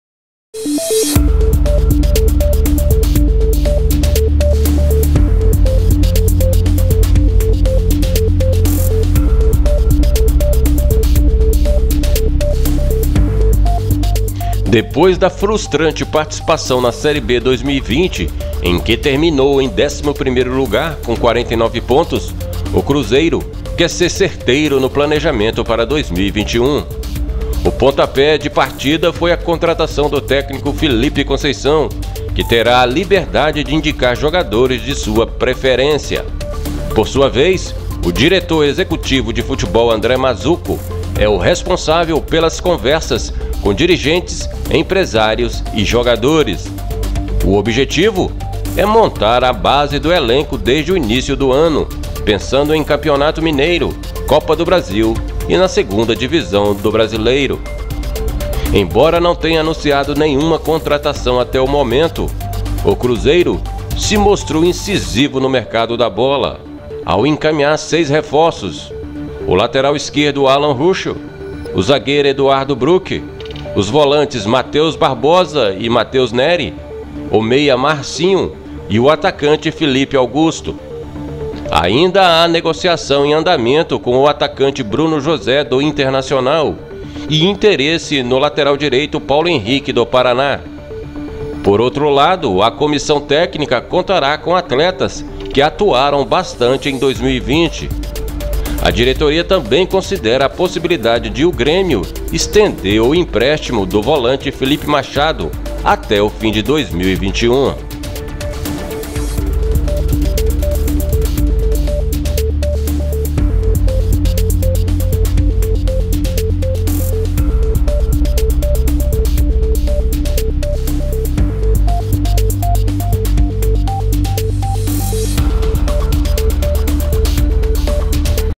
√ Cruzeiro I No mercado da bola I Temporada 2021 I 12.02.2021
√ CRUZEIRO
Cruzeiro inicia temporada 2021 com movimentação intensa no mercado da bola. Clube pretende montar base do elenco desde o início do estadual.

Depois da frustrante participação na Série B 2020, em que terminou em 11º lugar, com 49 pontos, o Cruzeiro quer ser certeiro no planejamento para 2021. O pontapé de partida foi a contratação do técnico Felipe Conceição, que terá a liberdade de indicar jogadores de sua preferência. Por sua vez, o diretor executivo de futebol, André Mazzuco, é o responsável pelas conversas com dirigentes, empresários e jogadores. O objetivo é montar a base do elenco desde o início do ano, pensando em Campeonato Mineiro, Copa do Brasil e na segunda divisão do Brasileiro. A estreia no estadual será no dia 28 de fevereiro (domingo), às 10h, contra o Uberlândia, no estádio Parque do Sabiá.

“Todas as três competições no nosso calendário deste ano são importantes por fatores distintos. A gente tem o critério técnico que precisamos disputar o título do Mineiro como foco. A Copa do Brasil, financeiramente, é muito importante e precisa ser dada a devida atenção.  Ao mesmo tempo, a Série B, um campeonato longo que nos dá o calendário do ano que vem, para o acesso à Série A”, afirmou Mazzuco, em entrevista ao Superesportes.

“Diferentemente do ano passado, vamos começar com uma equipe bem montada, com jogadores e remanescentes no elenco que nós queríamos. Vamos começar o trabalho com a equipe formatada. Naturalmente, ao longo da temporada, podem surgir oportunidades e outros acontecimentos de saída, mas a gente começa o trabalho com novo treinador e 14 dias de preparação para formatar as ideias e implementar essa filosofia que a gente deseja. Nossa ideia é participar do Mineiro com essa base montada e a equipe em formação para que a gente tenha uma temporada mais sólida”, complementou o dirigente.

Embora não tenha anunciado nenhuma contratação até o momento, o Cruzeiro se mostrou incisivo no mercado da bola ao encaminhar seis reforços: o lateral-esquerdo Alan Ruschel, o zagueiro Eduardo Brock, os volantes Matheus Barbosa e Matheus Neris, o meia Marcinho e o atacante Felipe Augusto. Há negociação em andamento com o atacante Bruno José, do Internacional, e interesse no lateral-direito Paulo Henrique, do Paraná - segundo o Blog da Nadja, do Globoesporte, o Juventude é o destino do jogador.

A estratégia da Raposa também passa pelas saídas do elenco de 2020. O lateral-direito Rafael Luiz, que era reserva de Raúl Cáceres, foi requisitado pela Ferroviária-SP, enquanto o lateral-esquerdo Patrick Brey, com contrato até março, não permanecerá na Toca. O clube ainda vendeu o volante Jadsom Silva ao Bragantino, por R$5,4 milhões; emprestou o atacante Sassá ao Marítimo, de Portugal; e deve abrir mão do zagueiro Cacá, que tem sondagens do futebol japonês.

Por outro lado, a comissão técnica contará com atletas que atuaram bastante em 2020, como o goleiro Fábio, os zagueiros Manoel e Ramon e os atacantes Airton, Rafael Sobis e William Pottker. A diretoria também considera a possibilidade de o Grêmio estender o empréstimo do volante Filipe Machado até o fim de 2021.

E os salários?

Ao mesmo tempo em que anima a sua torcida com a reformulação do grupo de jogadores, o Cruzeiro convive com dificuldades financeiras e não estipula prazo para colocar os salários em dia. André Mazzuco explicou ao Superesportes que esse tema tem sido tratado de maneira bastante transparente tanto com os remanescentes da equipe de 2020 quanto com os que chegam à Toca em 2021.

“A gente procura ser muito transparente no cenário do clube. Temos um diálogo totalmente aberto e muito transparente com os jogadores que estão aqui conosco. Mostramos a eles o que a gente vai enfrentar e, ao mesmo tempo, a busca constante por soluções. Em momento algum dissemos a eles que seria fácil, porque não vai ser, mas temos uma equipe trabalhando para isso”.